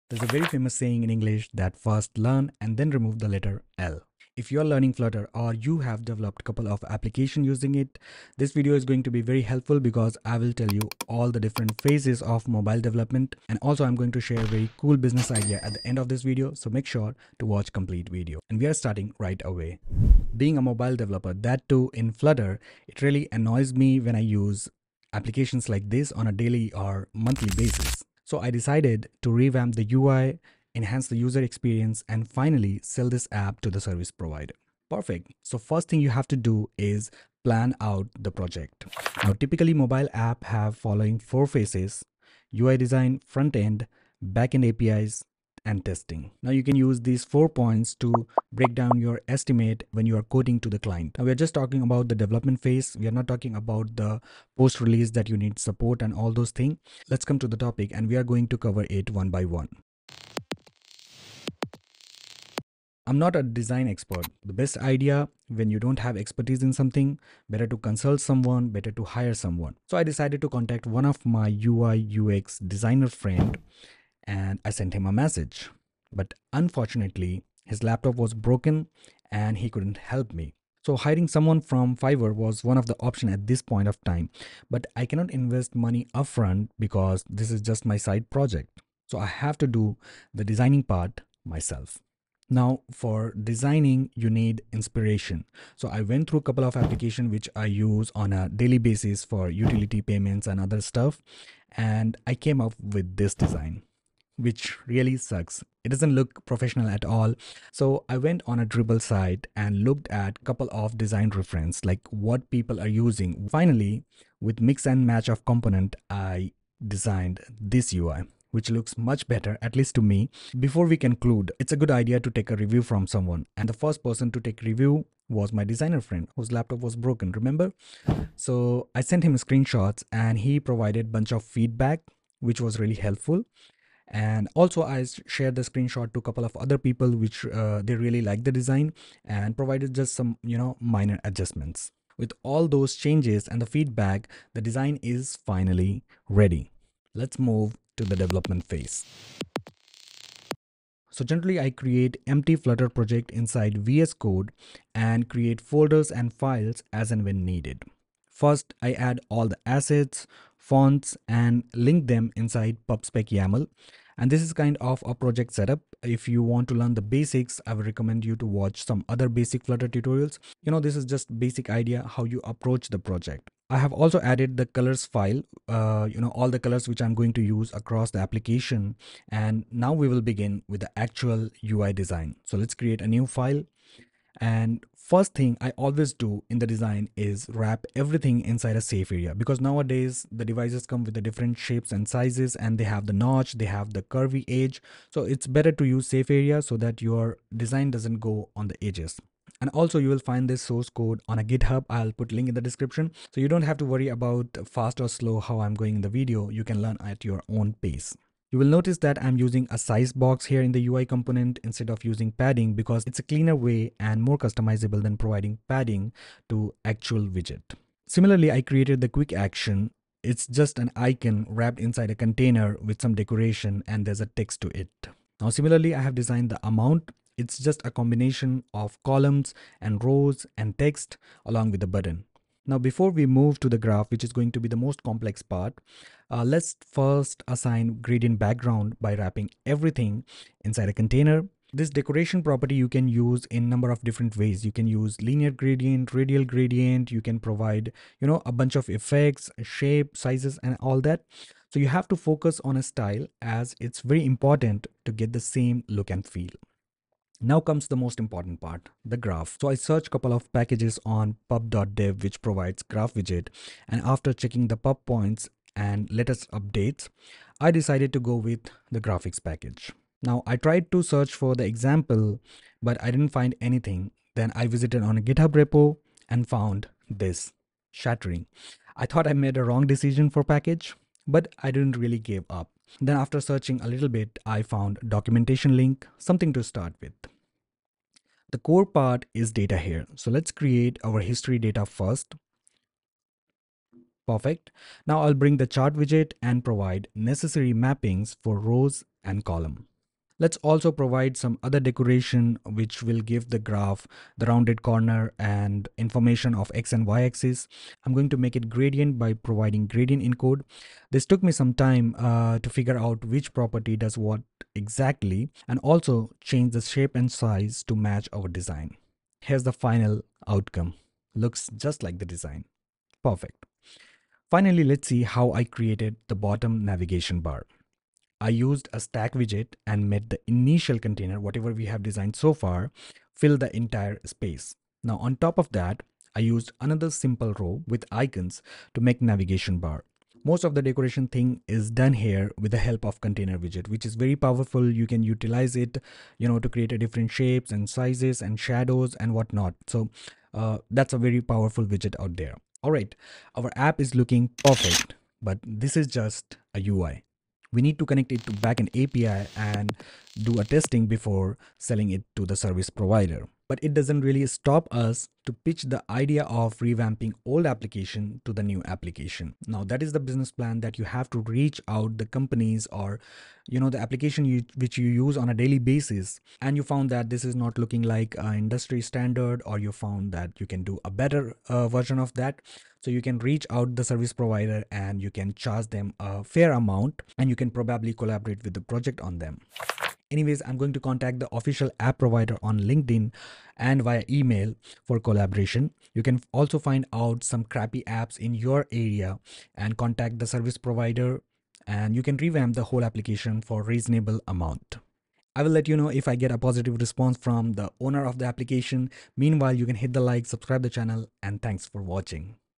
Mobile Development: From Concept to Business Success
Are you looking to turn your mobile app idea into a thriving business? In this video, we break down the essential phases of mobile development, from the initial concept and design to development, testing, and launching.

Learn how each stage contributes to creating a successful mobile app and discover strategies to monetize and market your app effectively. Whether you're a budding entrepreneur or an experienced developer, this guide will provide you with the insights and tools needed to turn your mobile app into a profitable venture.

Gears
Camera 📸 || Canon 250D
Mic 🎤 || Maono AU-04